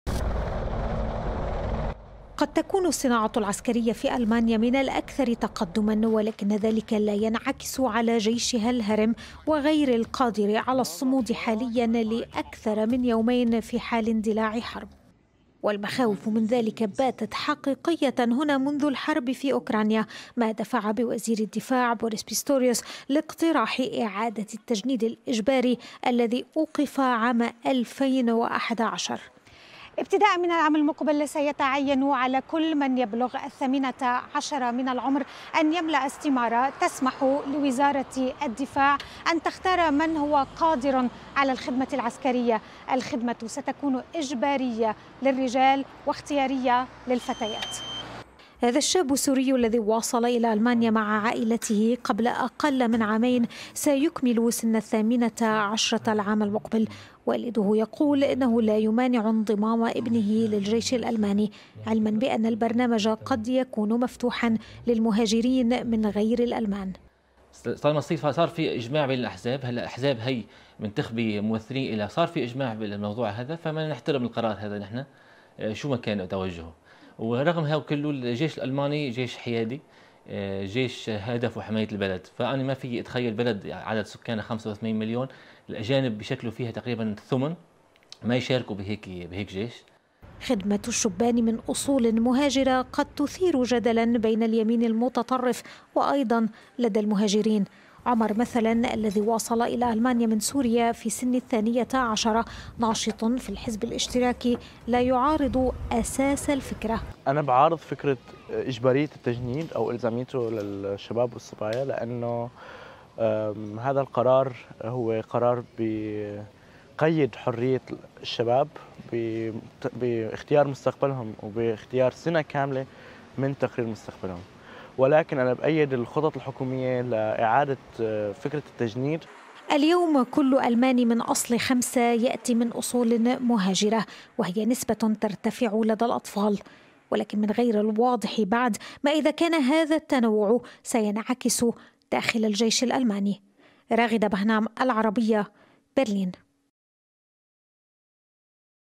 جيش لا يصمد ليومين.. ألمانيا تعيد التجنيد الإجباري للشبان وسط مخاوف أمنية
إلزامي للشبان واختياري للفتيات.. ألمانيا تجبر كل من يبلغ سن الـ 18 على التجنيد وقد تطال الخطوة المهاجرين
لمعرفة أحدث المستجدات.. عبر البث المباشر من هنا 👇
وبإمكانكم متابعة العربية على منصات التواصل